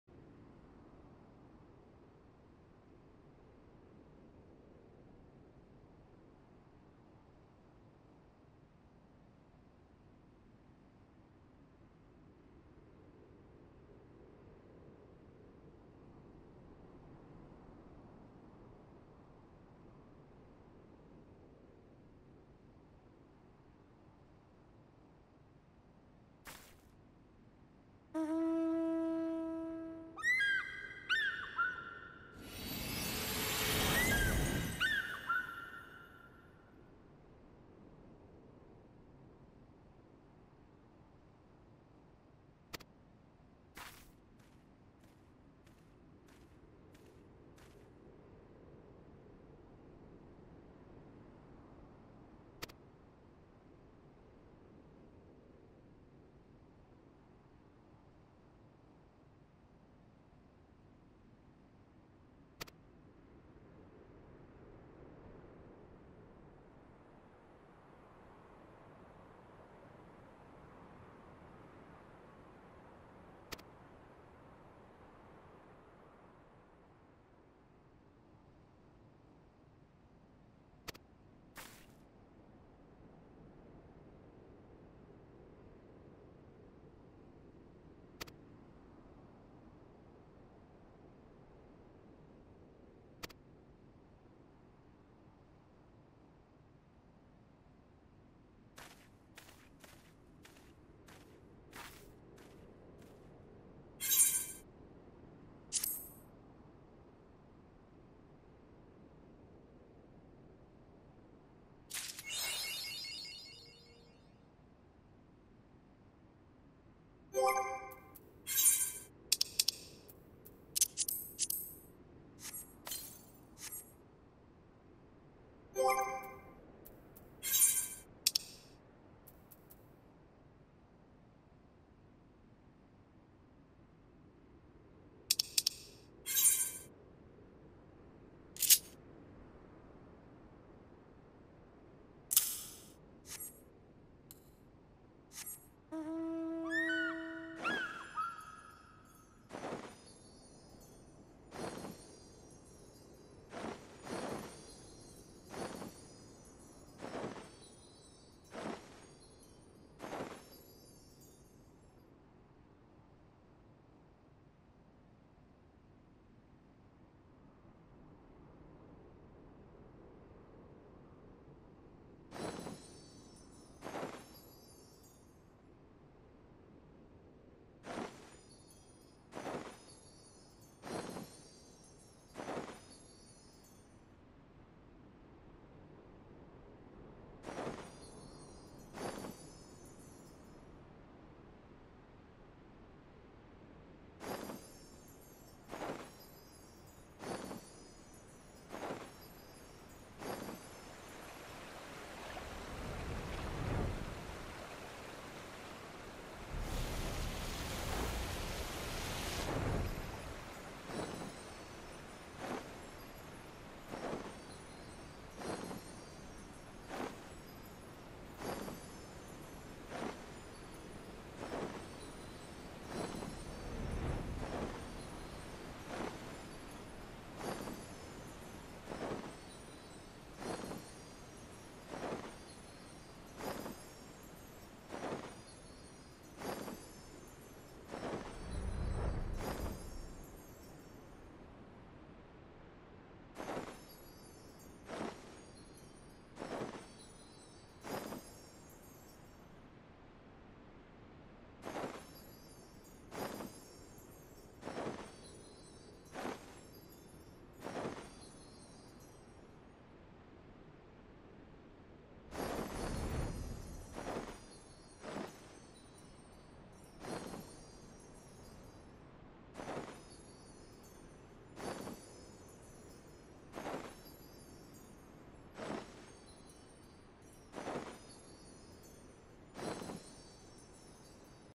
Finaly Got My Fire Bird
After many hours of grinding, I finally got my firebird!